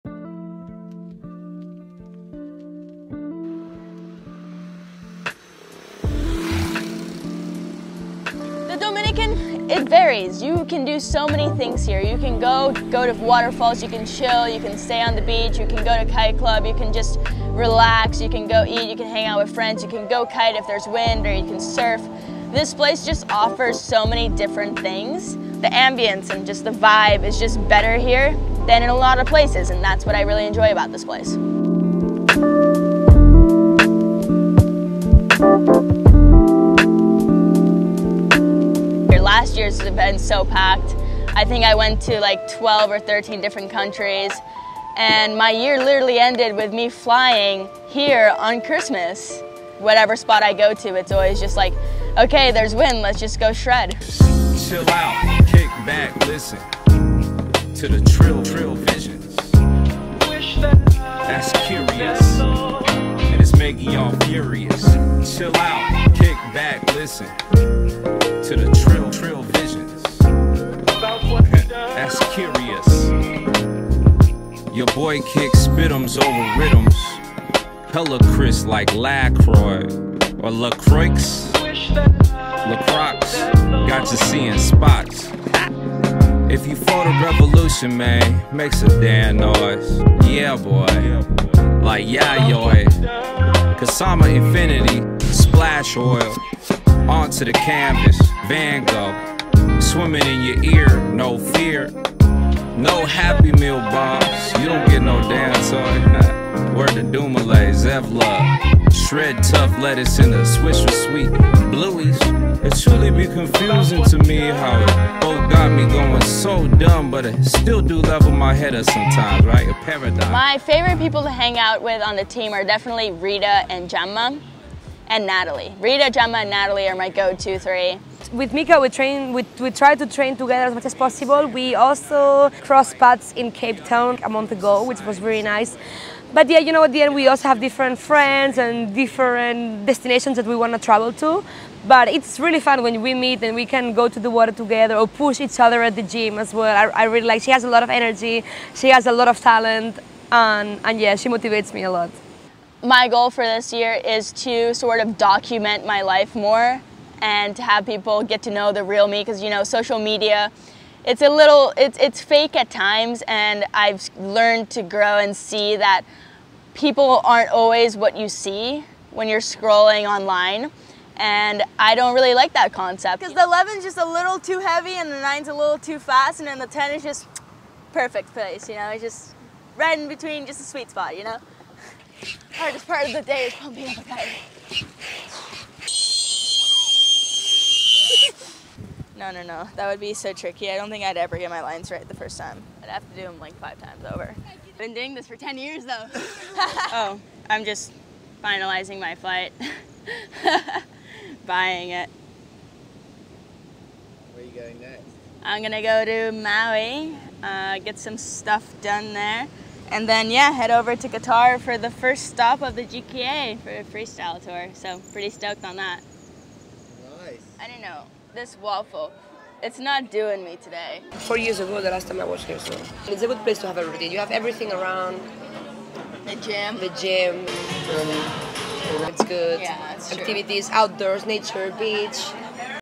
MIKAILI - Dominican Republic
In this first episode of 'MIKAILI' we follow the current World champion and arguably the most talented female Freestyle rider of all time, out to the Dominican Republic. We catch up with her at the end of the of the GKA off season. After her 6th world championship last season, she shreds across all kiteboarding disciplines... which ones will she choose for the 2023 season?

In this series, we will follow Mika's progress throughout her year. "Keeping it real" with how she thinks about her journey, what is getting her exited in the sport right now and who she is enjoying riding with. We manage to catch her, before she heads off to Maui and then on the Qatar for the first stop of the @GKAKITEWORLDTOUR.

Filmed and edited by: @tomcourtkite
Produced by: @DUOTONEKiteboarding